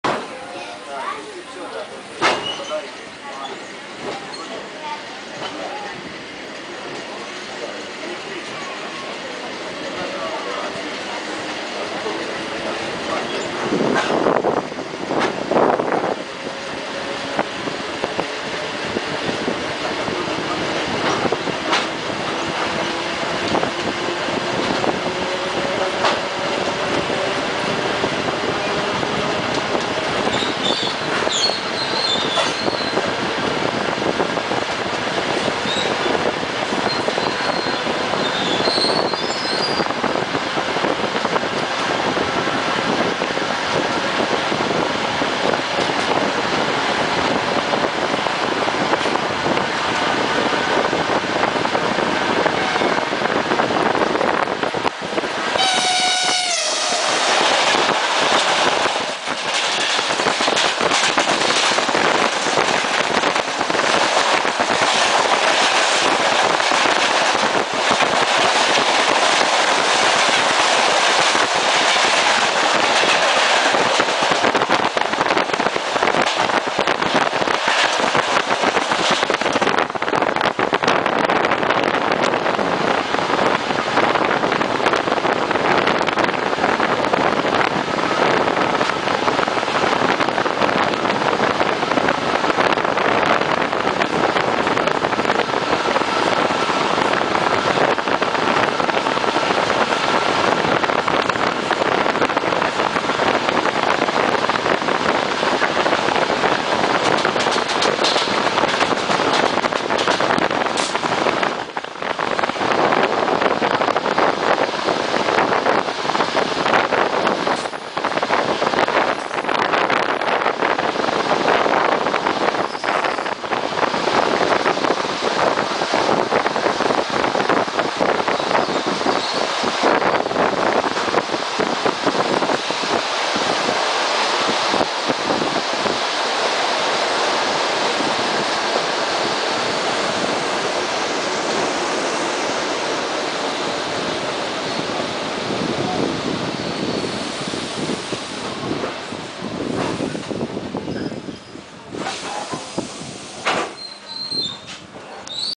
Лопатинці – Дубки. Електропоїзд ЕР9М-519 №6321 Жмеринка – Гречани. Частина 05. 14.08.2014
Украинские железные дороги. Пригородный электропоезд ЭР9М-519 №6321 Жмеринка – Гречаны. Часть 05. Лопатинцы – Дубки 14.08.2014
Лопатинці – Дубки. Електропоїзд ЕР9М-519 №6321 Жмеринка – Гречани. Частина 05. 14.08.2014. Українські залізниці.